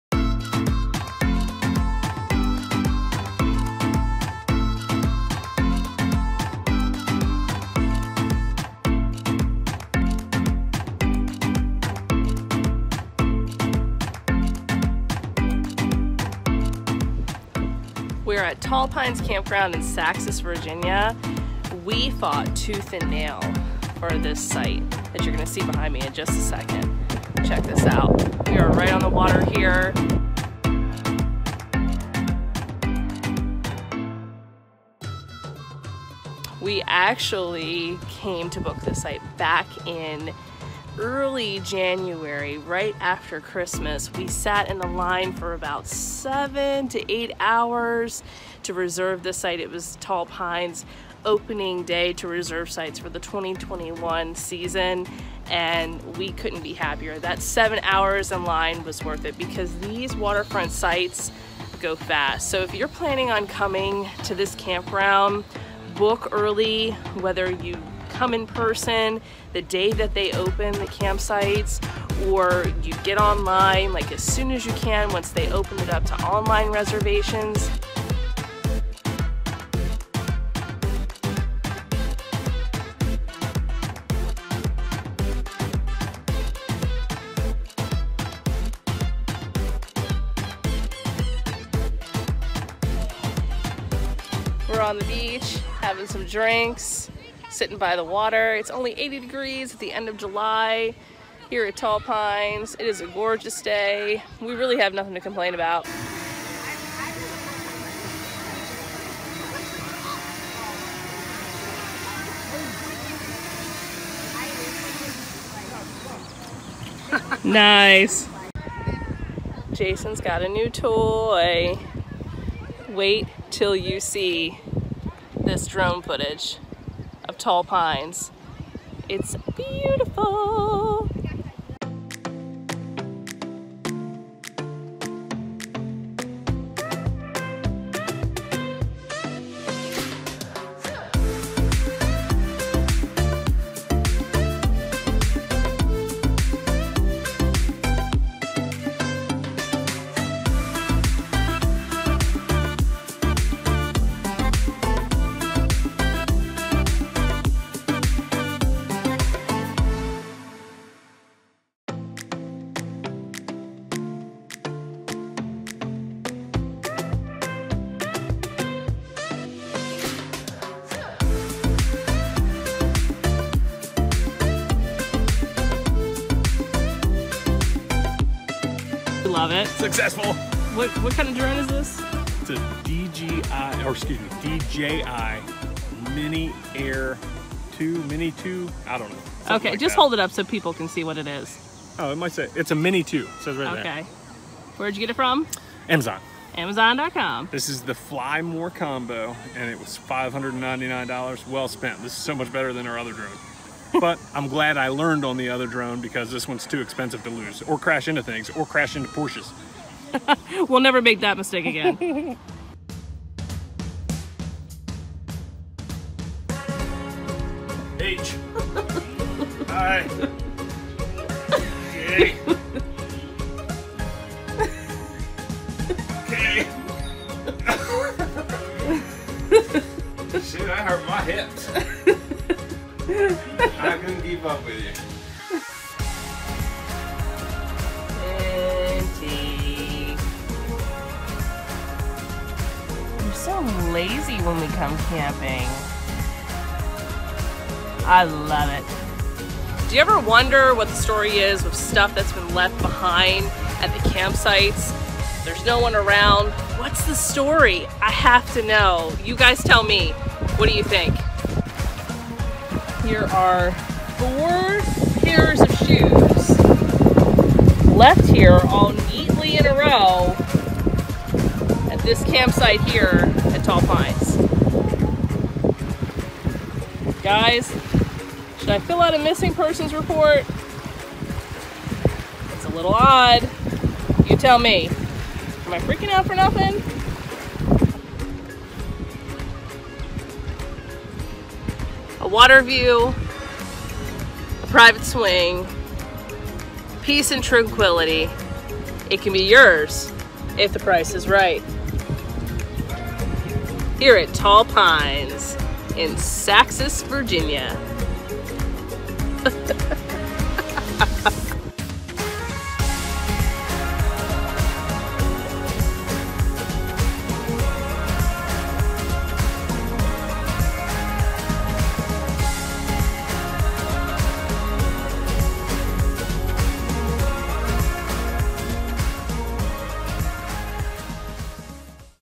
New DJI Drone takes FLIGHT at Tall Pines!
We show off our waterfront campsite at Tall Pines Harbor Campground near Saxis, Virginia. While there, our new DJI Mini 2 drone takes its first flight!